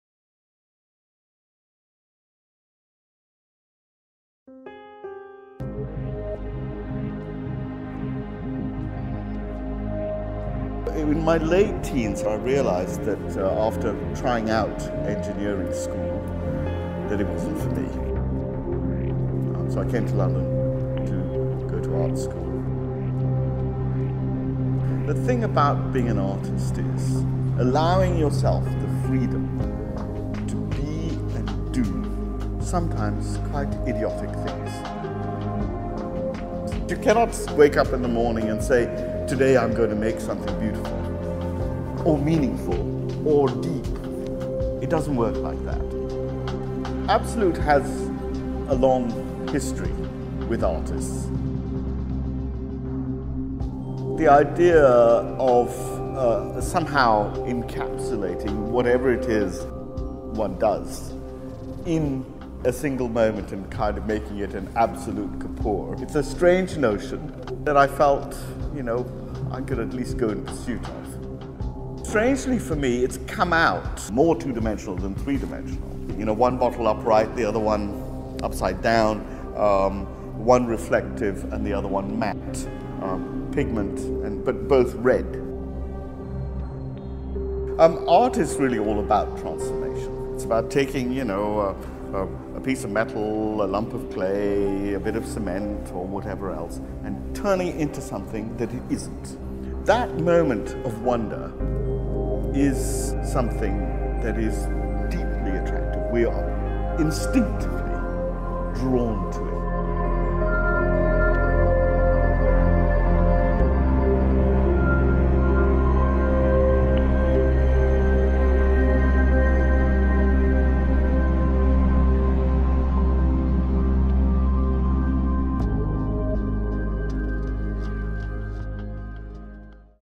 ABSOLUT KAPOOR - An installation by Indian artist Anish Kapoor in collaboration with Absolut.
An installation by Indian artist Anish Kapoor in collaboration with Absolut will be unveiled during the India Art Fair in New Delhi, occurring from January 30 to February 2, 2014.
The ABSOLUT KAPOOR piece comes across as the negative of a photograph through the use of a vibrant color as its distinctive signature, and aims to bring together elements that redefine and transform the relationship between the object, the viewer, sculpture and space. Kapoor for Absolut wants to reinterpret the iconic bottle while evoking a sublime experience and generate amazement.